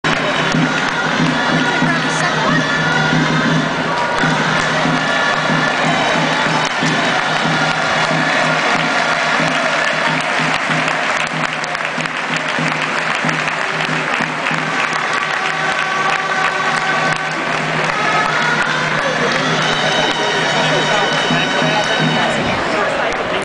BAR CAM 07- HUMAN PYRAMID PART TWO
Crowd goes wild as top child is pulled onto the balcony!